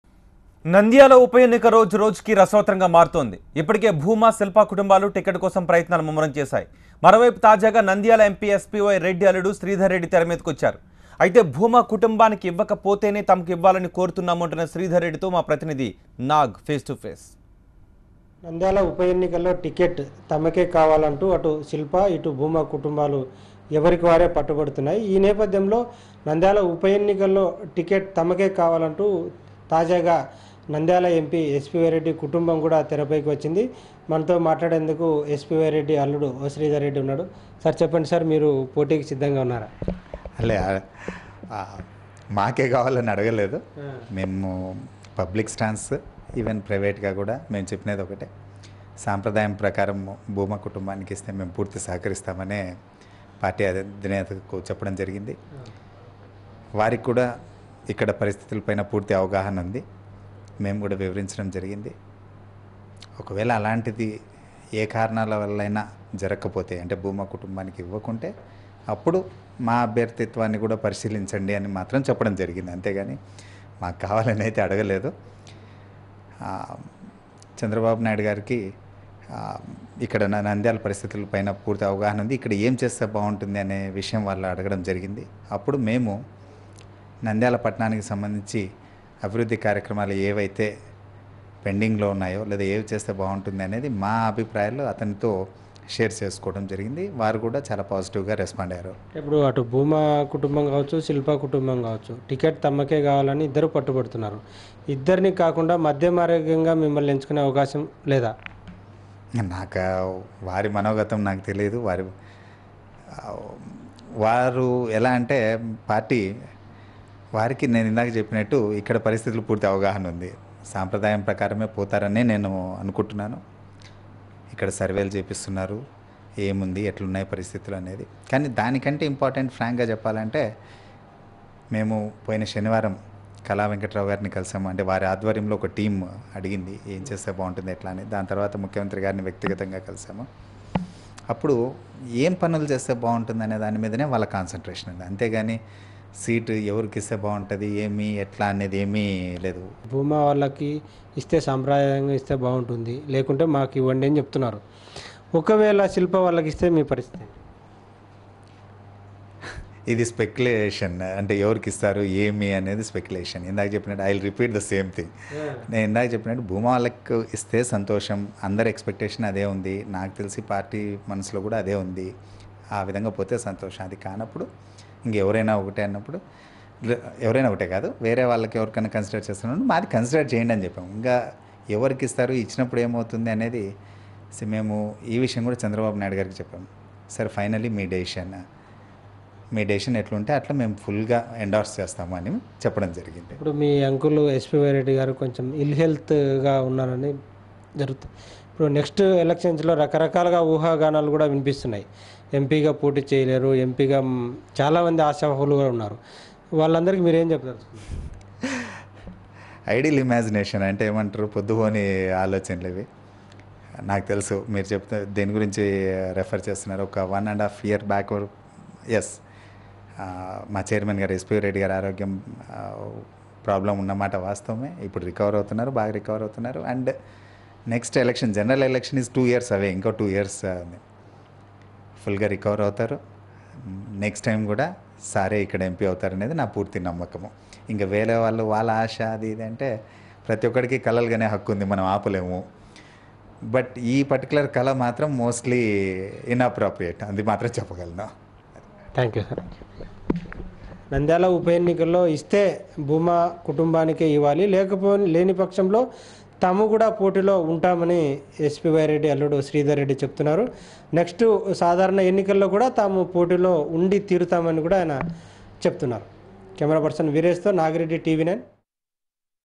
Nandyala Bypoll : MP SPY Reddy son-in-law not in race - TV9
Nandyala Bypoll : MP SPY Reddy son-in-law not in competition with Bhuma family